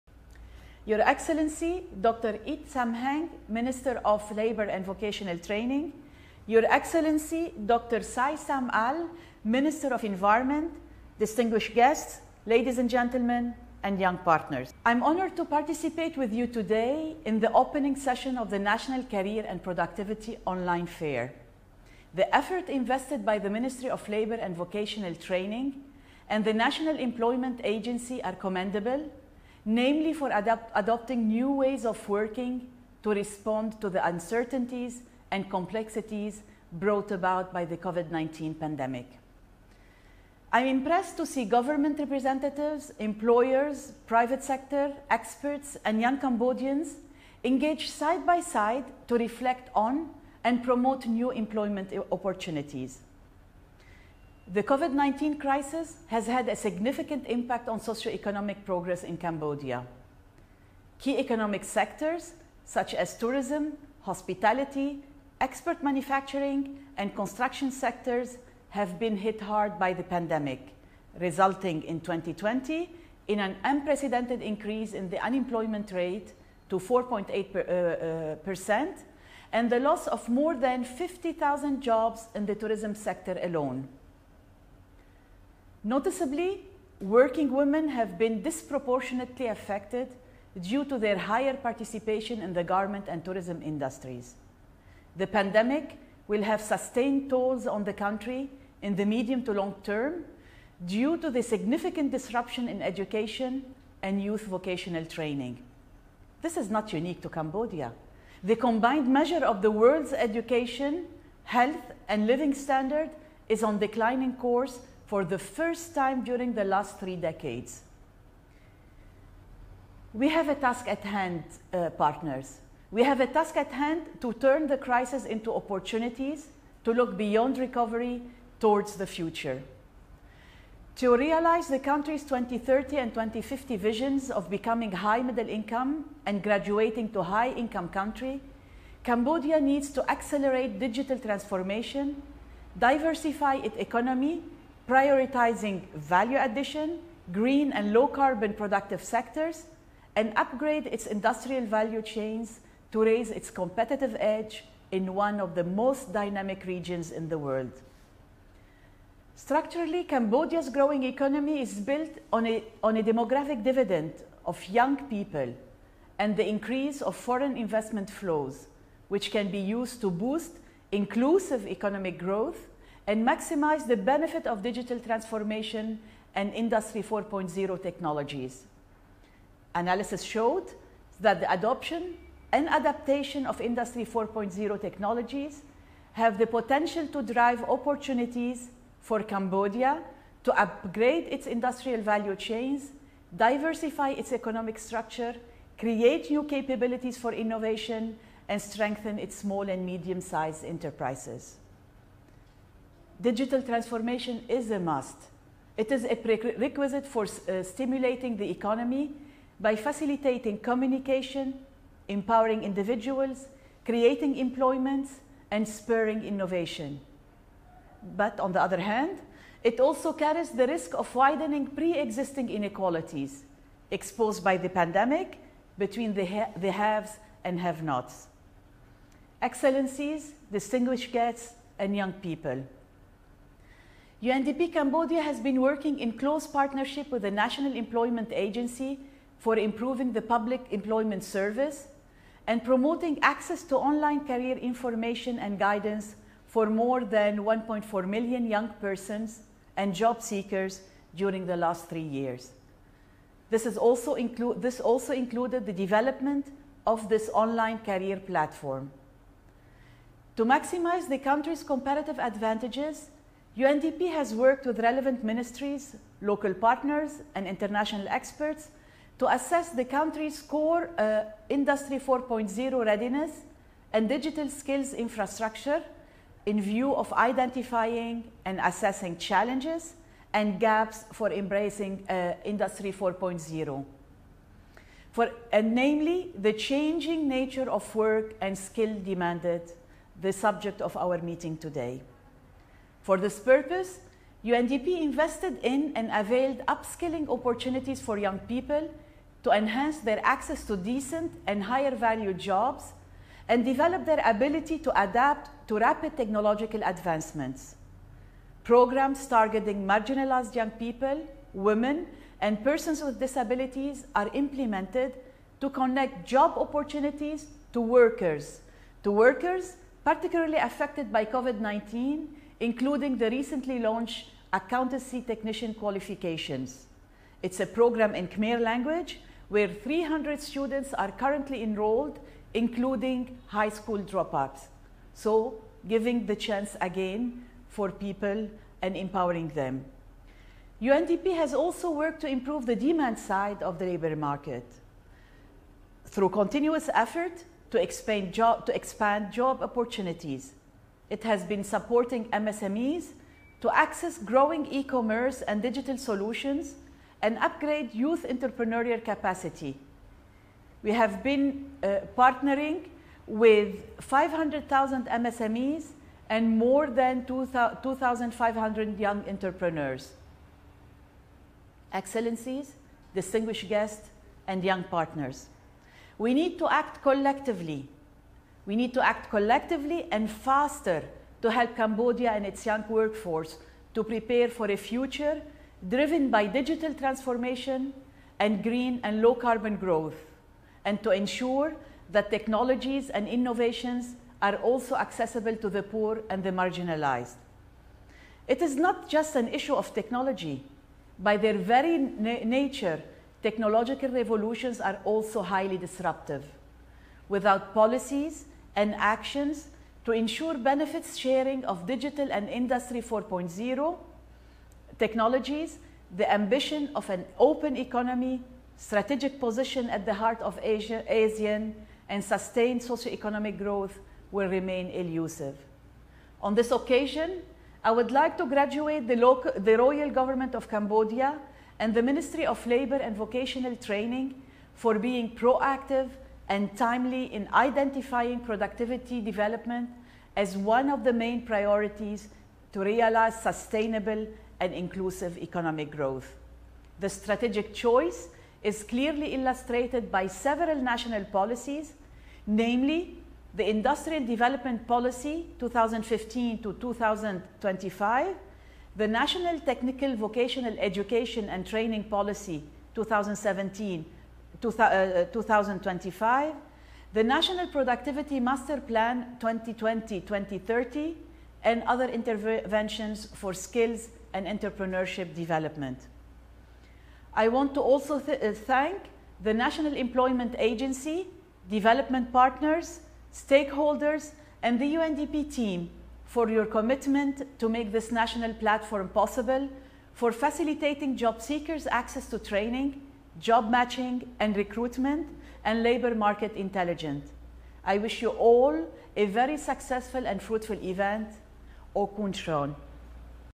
លោកស្រី Alissar Chaker អ្នកតំណាងប្រចាំប្រទេសនៃកម្មវិធីរបស់អង្គការអភិវឌ្ឍន៍សហប្រជាជាតិ (UNDP)
សុន្ទរកថាស្វាគមន៍ដោយ លោកស្រី Alissar Chaker អ្នកតំណាងប្រចាំប្រទេសនៃកម្មវិធីរបស់អង្គការអភិវឌ្ឍន៍សហប្រជាជាតិប្រចាំប្រទេសកម្ពុជា (UNDP)